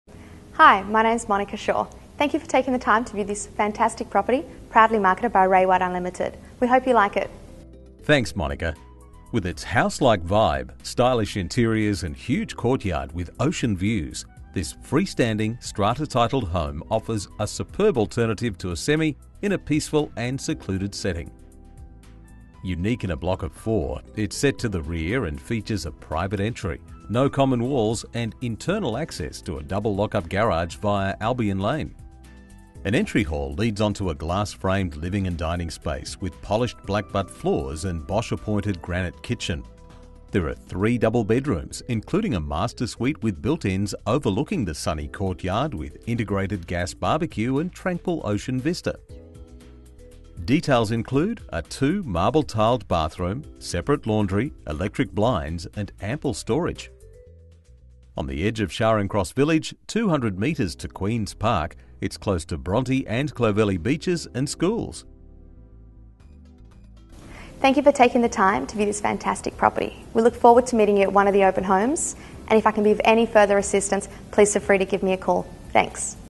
SOLD UNDER THE HAMMER! - 4/3 Macpherson St, Bronte
With its house-like vibe, sleek interiors and huge courtyard capturing ocean views, this freestanding strata-titled home offers a superb alternative to a semi in a quiet and secluded setting. Set to the rear of a block of four, it features a private entry(via Albion Lane), no common walls and internal access to a double lock-up garage.

• Sleek glass-framed living/dining
• Blackbutt floors, wraparound terrace
• Sleek Bosch/granite kitchen
• 3 double beds, 2 marble-tiled baths
• Large master suite with built-ins
• Sunny bbq courtyard, sea breezes
• Internal access to DLUG, storage
• Stroll to cafes, 200m to Queens Park

Auction: Wednesday 10 September, 6.30pm
Club Rose Bay
Cnr Vickery Avenue & NSH Road
Parking behind Lyne Park Tennis

Agent: Monica Shor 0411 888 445